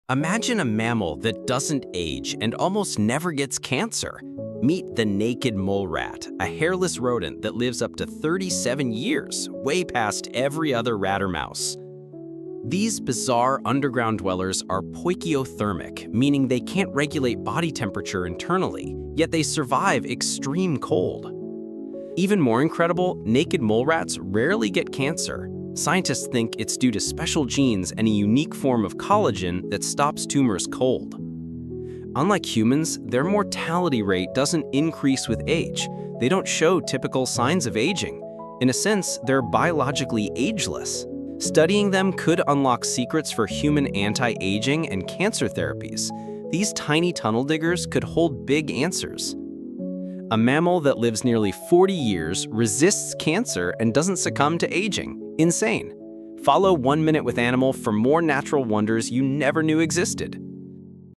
This Mammal Doesn’t Age Like We Do 😳🐀
The naked mole-rat can live up to 37 years, resists cancer, and doesn’t show signs of aging.
Scientists say it's the closest thing to an ageless mammal ever discovered.
Follow @OneMinuteWithAnimal for more amazing true animal stories.

📚 Sources: BBC Science Focus, NIH, Scientific American, Nature